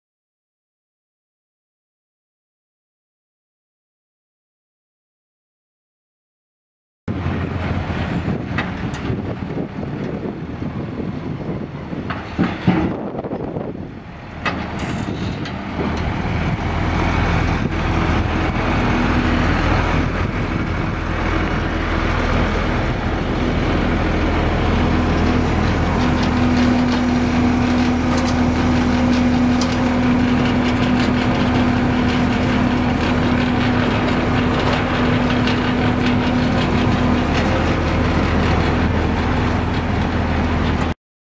1992 International 4900 semi truck for sale | sold at auction September 26, 2013
1992 International 4900 semi truck
111,717 miles on odometer
6,714 hours on meter
Twin screw
Heat
12&#039; dump bed
Tailgate included
Flink 11&#039; plow
Flink 12&#039; stainless steel spreader
Pintle hitch
PTO
Spring suspension
Air brakes
170" wheelbase
11R22.5 tires

It was located in Auburn, KS, and we sold it to the highest bidder for $6,710.